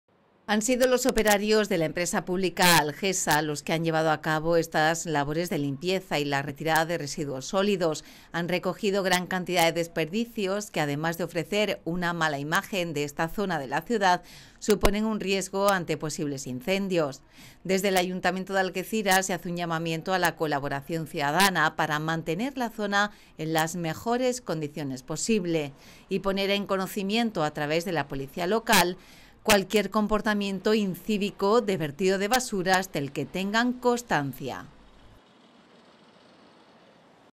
El Ayuntamiento limpia los arcenes de la N-350 y retira gran cantidad de residuos sólidos
El Ayuntamiento de Algeciras, a través la Delegación de Limpieza, está actuando esta semana en los márgenes de la carretera nacional N-350, a su paso por la barriada de El Saladillo. Se encargan de recoger la basura en esta zona que supone un riesgo ante posibles incendios.